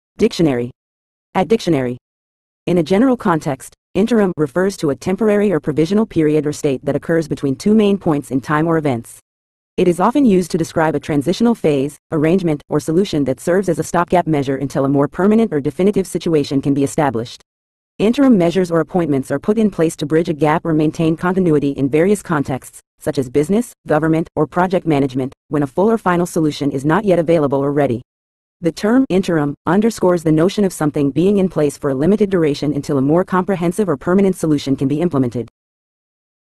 Interim Meaning In English
In a general context, "interim" refers to a temporary or provisional period or state that occurs between two main points in time or events. It is often used to describe a transitional phase, arrangement, or solution that serves as a stopgap measure until a more permanent or definitive situation can be established. Interim measures or appointments are put in place to bridge a gap or maintain continuity in various contexts, such as business, government, or project management, when a full or final solution is not yet available or ready. The term "interim" underscores the notion of something being in place for a limited duration until a more comprehensive or permanent solution can be implemented.